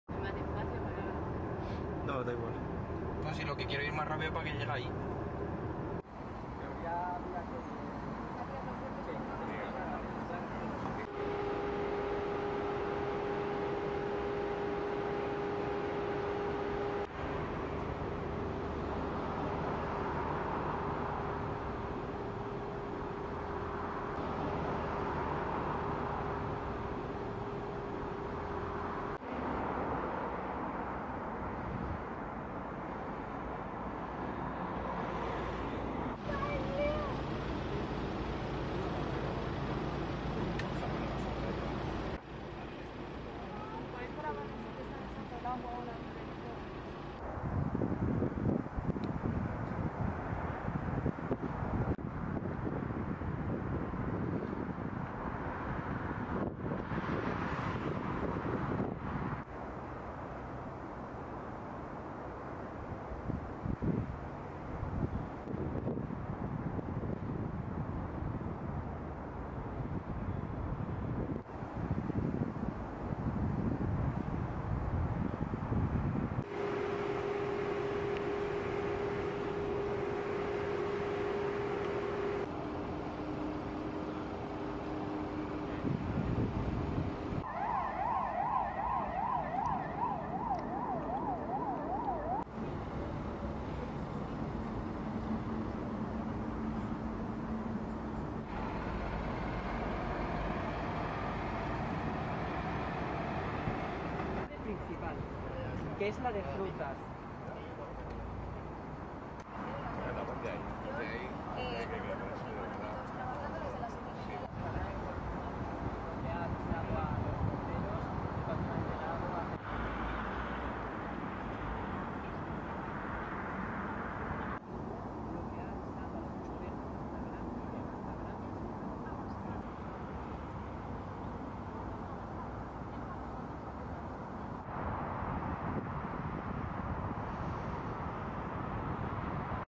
En büyük toptan satış pazarı yandı
İspanya'nın en büyük toptan satış pazarı olan "Mercamadrid'de yangın çıktı. 5 bin metrekare alanda bulunan, 11 büyük meyve şirketi kullanılamaz hala geldi. Yangın Cumartesi gece 23.00 sıralarında kontrol altına alındı. Yangında toplam dört depo yanarken, ölü veya yaralının olmaması sevindirdi.